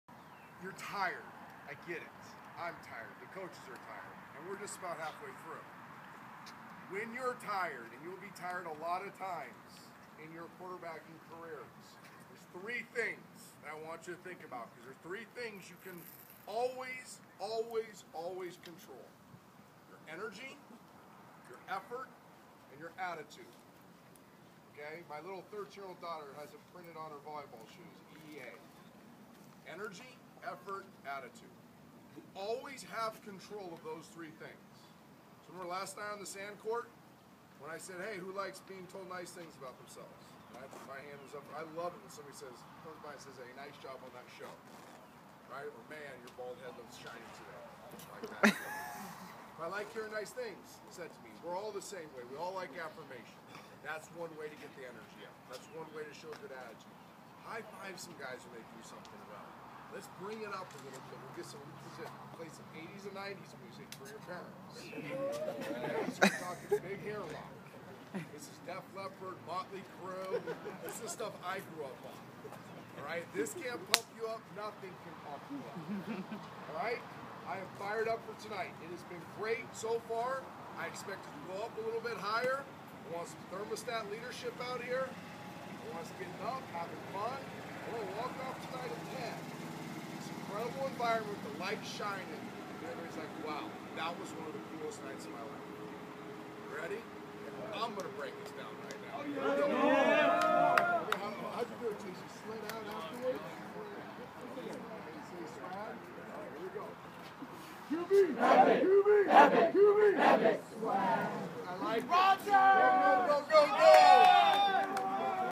Energy, effort and attitude. - @TDESPN June 20, 2015
Trent Dilfer speaks on the responsibilities of a great QB. @qbcentric QBEpic @LakePointSports